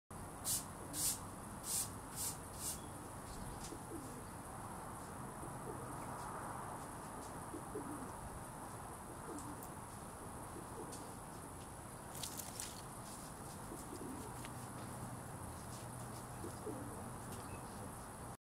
gollum feeding a mouse
Gollum helps to feed the wildlife in the garden.